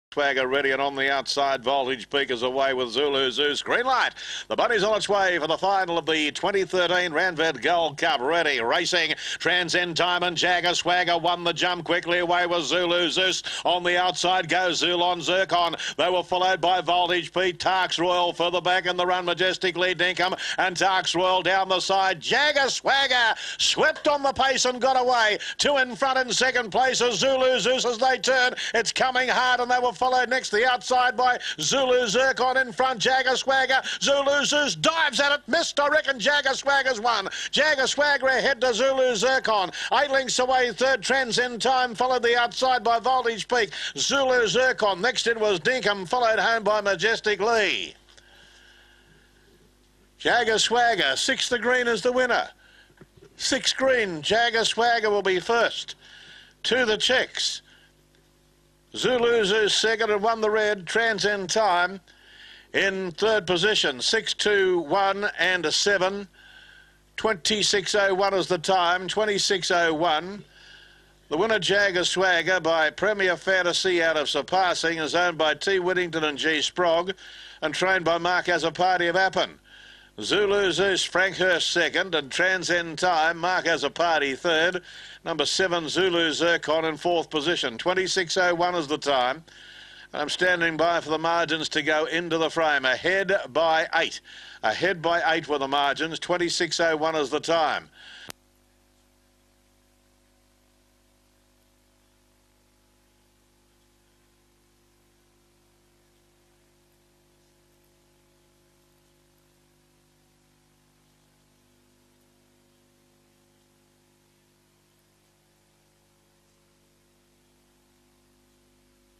2013 Bulli Gold Cup
Jagger Swagger Wins The Bulli Gold Cup In 2013